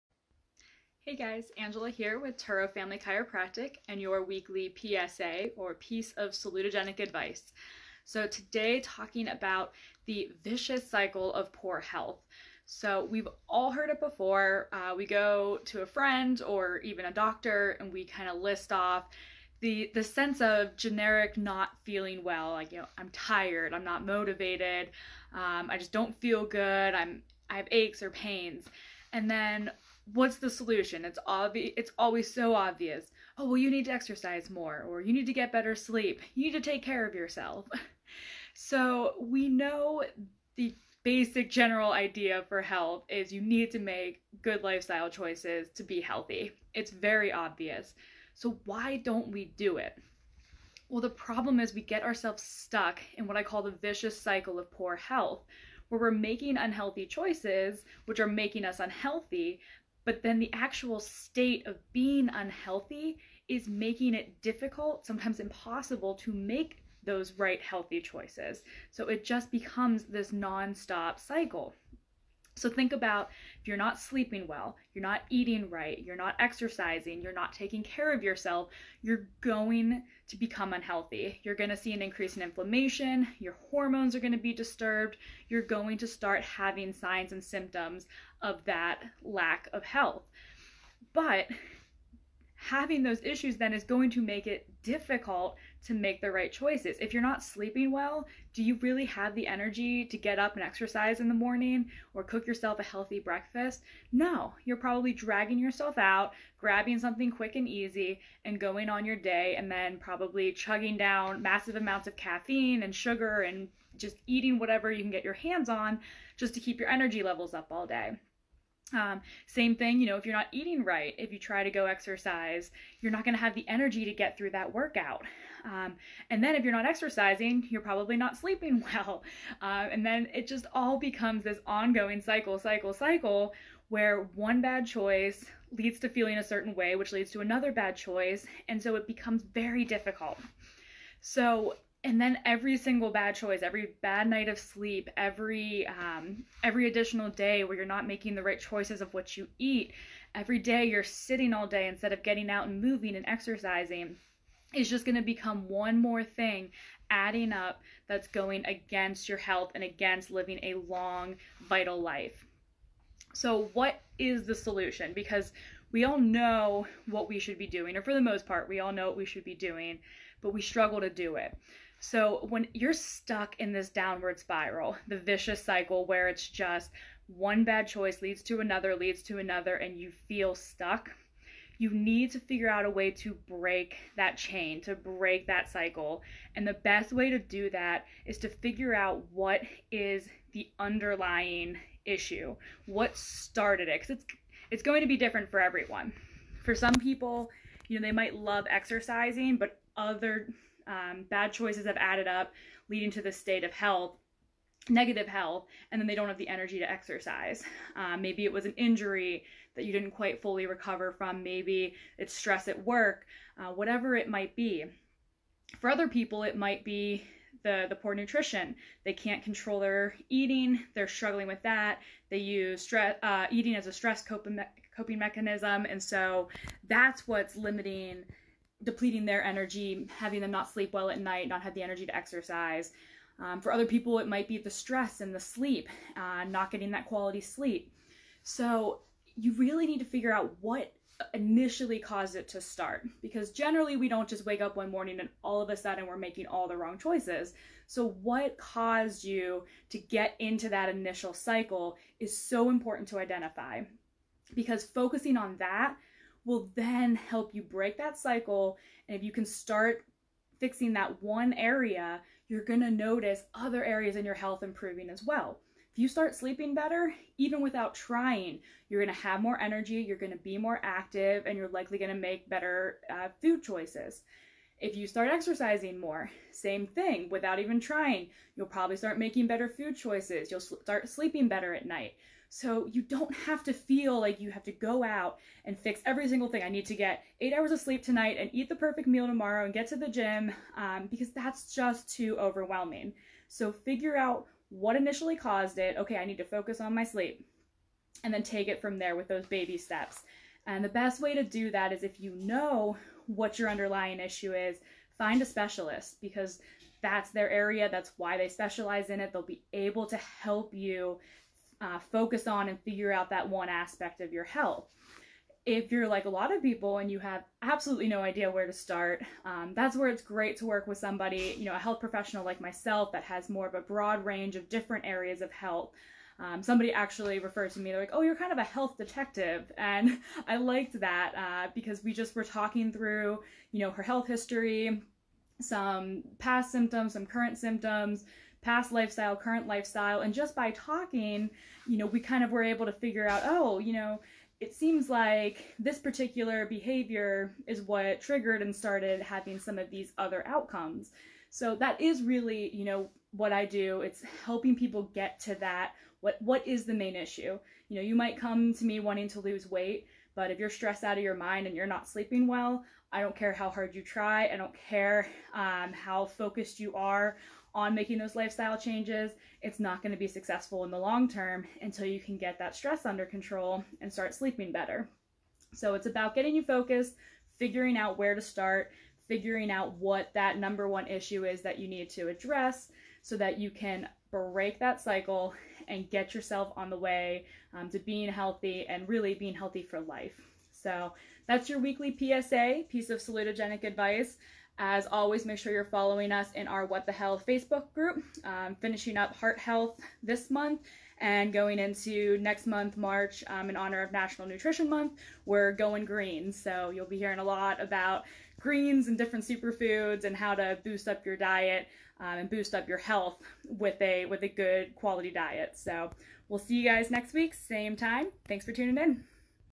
The Vicious Cycle of Poor Health
This week's PSA (Piece of Salutogenic Advice) with Angela: The Vicious Cycle of Poor Health. Once the wheel is turning, sometimes it's hard to make it stop. But you can!